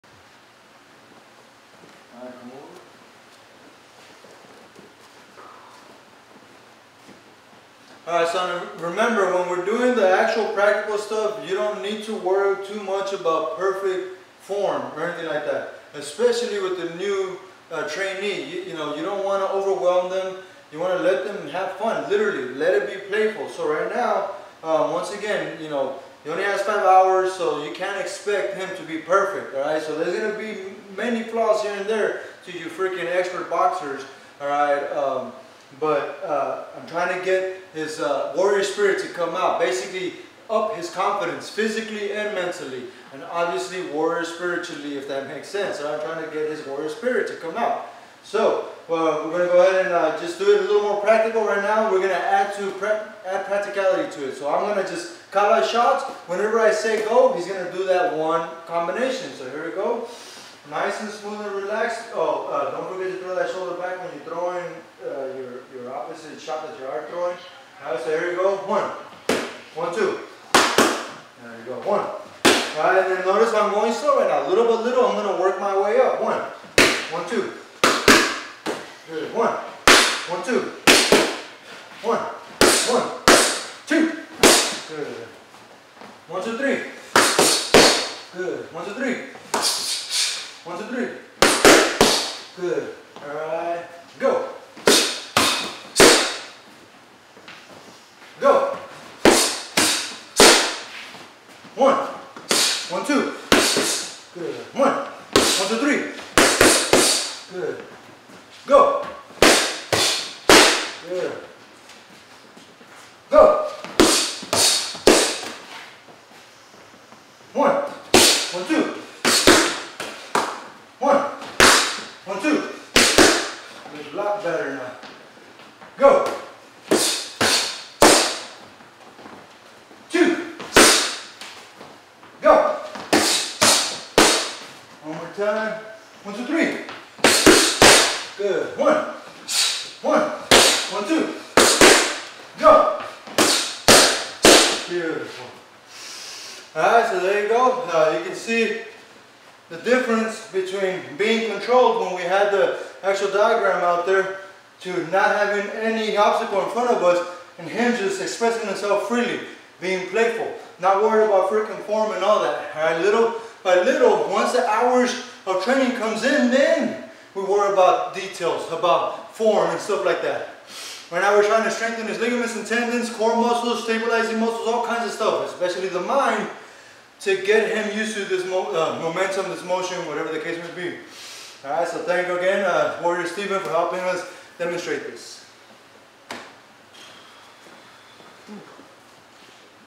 Reviewing Combination Ability & Reflexes on New Student
Here is a video that shows how to drill the necessary foot work to accomplish such movements you see in the counter being drilled in this video.